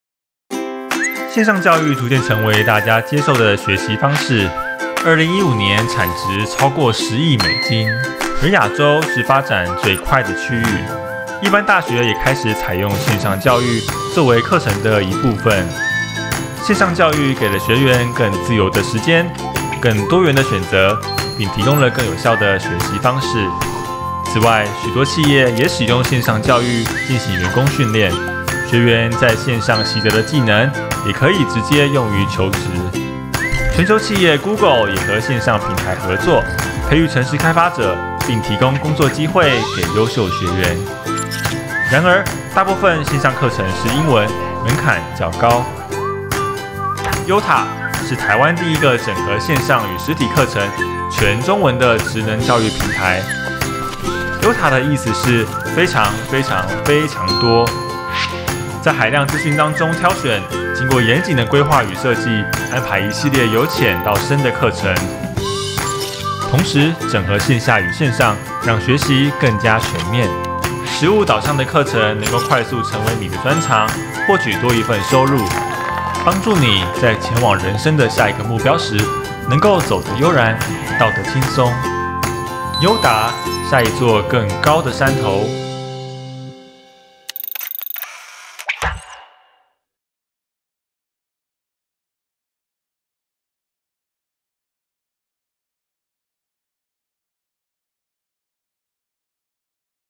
YOTTA│整合線上與實體課程，創造嶄新學習體驗
“自我能力的全方位提升”
YOTTA是來自臺灣，全中文的跨域教育平台。
致力於數位教學內容規劃並結合實體課程資源
虛實整合，創造嶄新學習體驗。